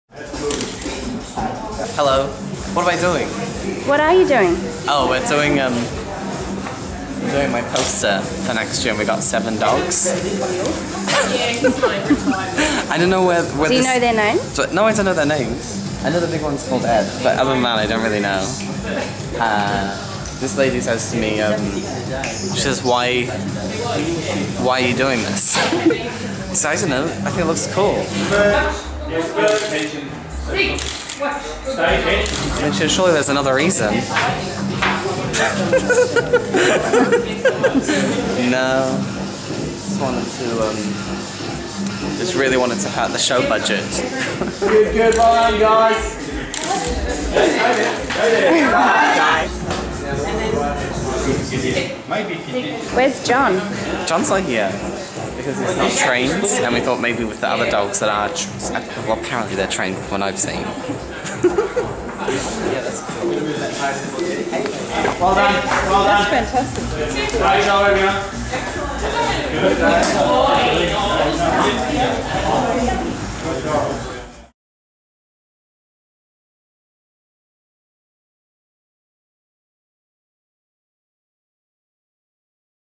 Josh Thomas 'Everything Ever' Poster Shoot
On set of Josh Thomas's photo shoot for the poster of his new live show 'Everything Ever'.